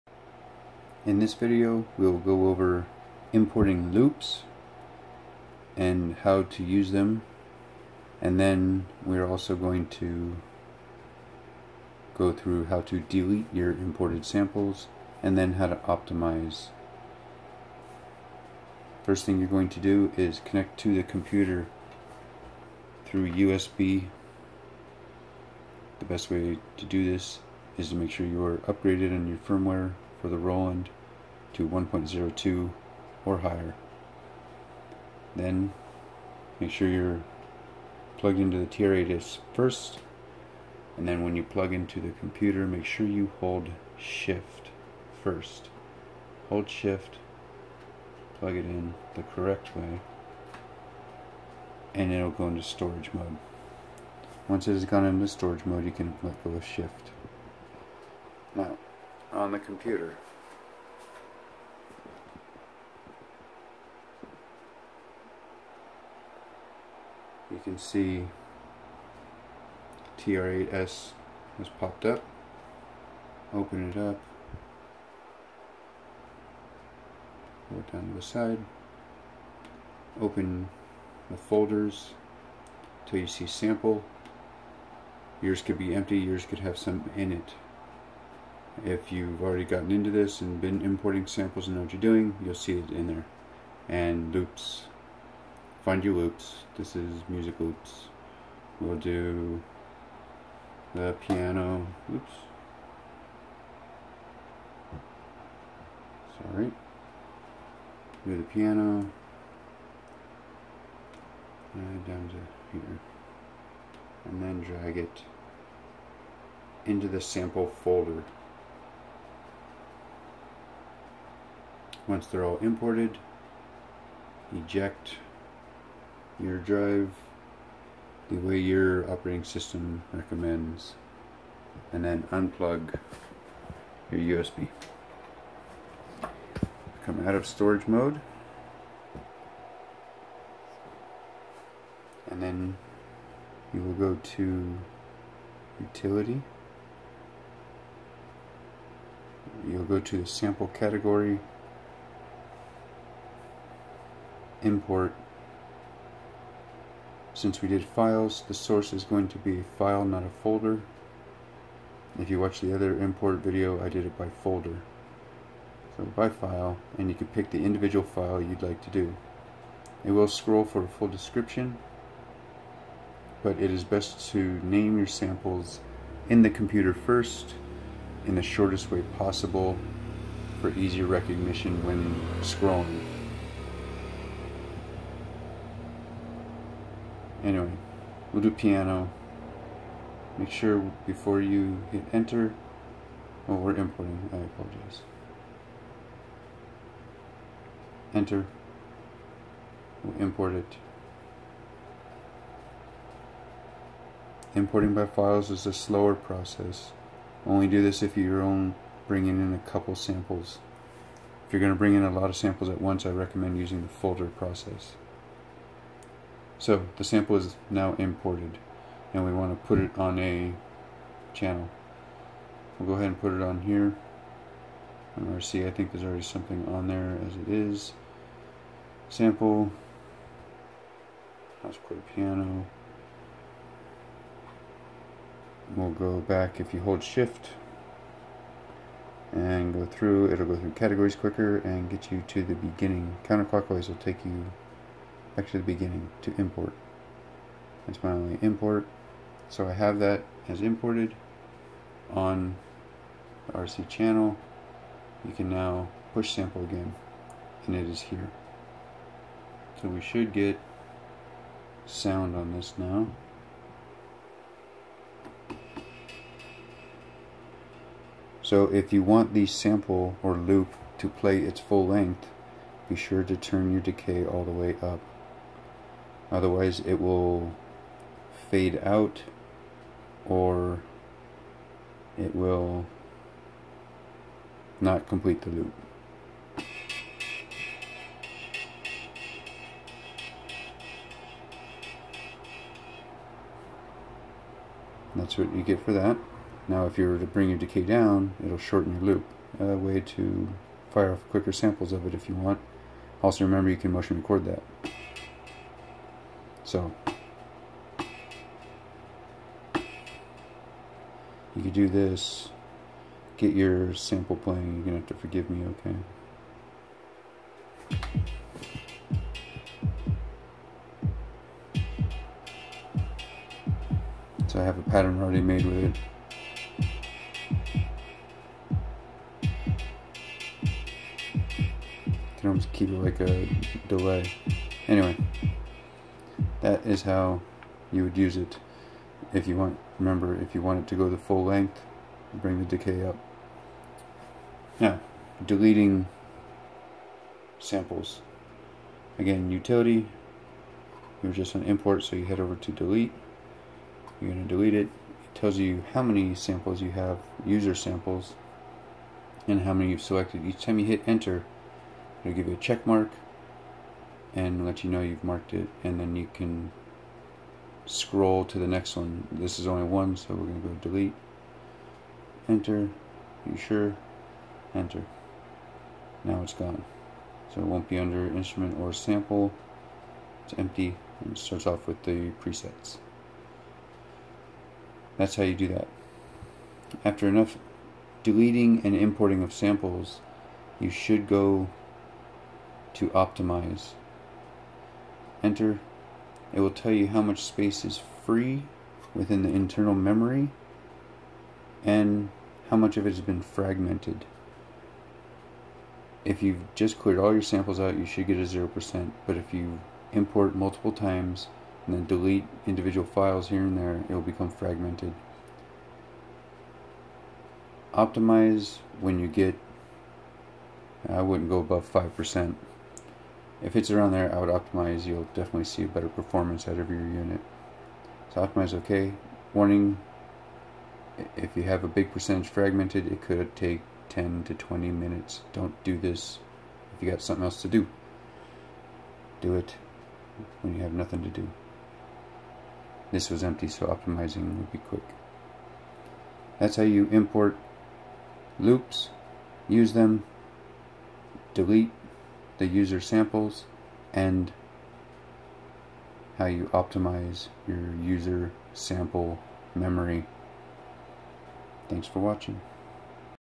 Roland TR-8s Tutorial: Importing Loops As Samples
Another importing video, but this one uses loops. Also learn a few tips on using loops, how to delete the sample and how to optimize user storage area.